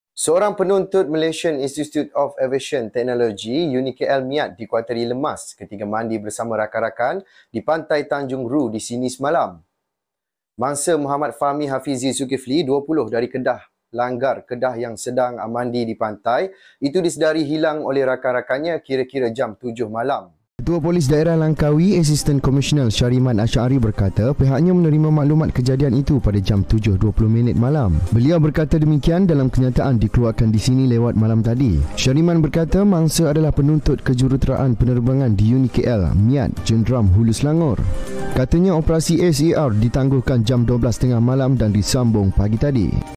Buletin Semasa | Penuntut "UNIKL MIAT MAUT" Dikhuatiri Lemas di Tanjung Dhu | Imedikel TV
SEORANG PENUNTUT MALAYSIAN INSTITUTE OF AVIATION TECHNOLOGY (UNIKL MIAT) DIKHUATIRI LEMAS KETIKA MANDI BERSAMA RAKAN DI PANTAI TANJUNG RHU DI SINI, SEMALAM.

MANGSA, MUHAMMAD FAHMI HAFIZI ZULKIFLI, 20, DARI LANGGAR, KEDAH YANG SEDANG MANDI DI PANTAI ITU DISEDARI HILANG OLEH RAKAN-RAKANNYA KIRA-KIRA JAM 7 MALAM.